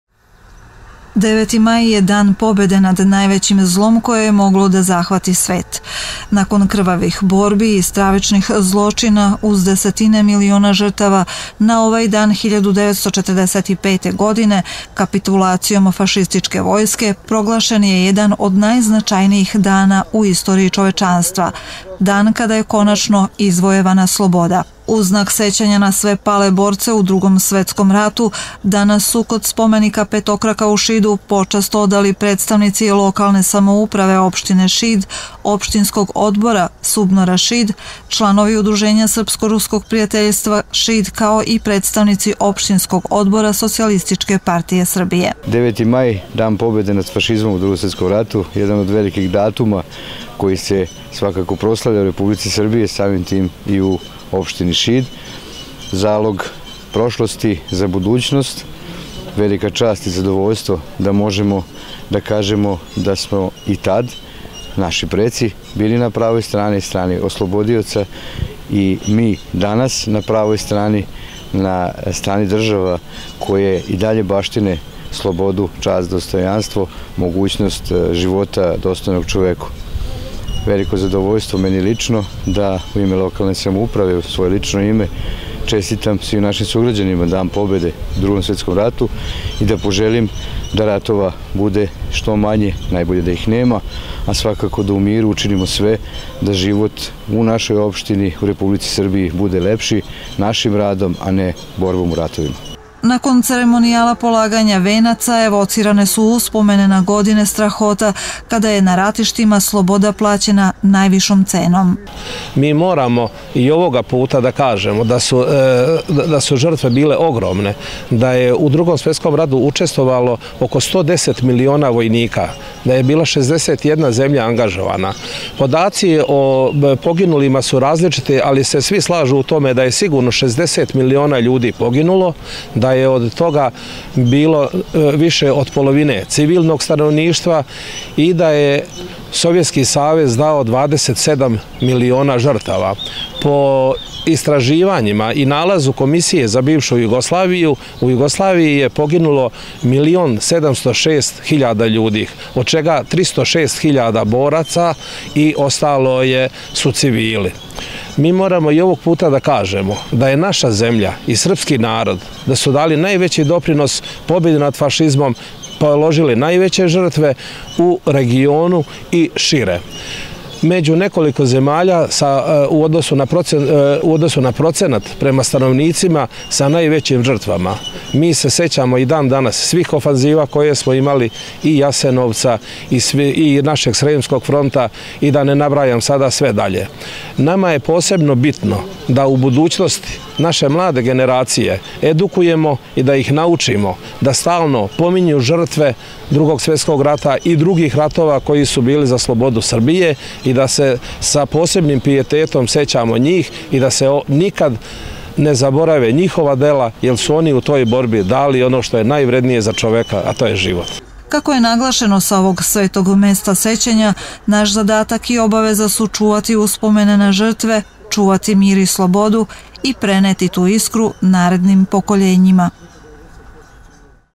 Dan pobede
Dan pobede nad fašizmom obeležava se danas u znak sećanja na 9. maj 1945. godine, kada je bezuslovnom kapitulacijom Nemačke okončan Drugi svetski rat u Evropi. Tim povodom danas su na mesnom groblju u Šidu pored spomenika Petokraka počast odali predstavnici lokalne samouprave, Opštiskog odbra SUBNORa Šid, Udruženja srpsko ruskog prijateljstva kao i članovi Opštinskog odbora socijalističke partije Srbije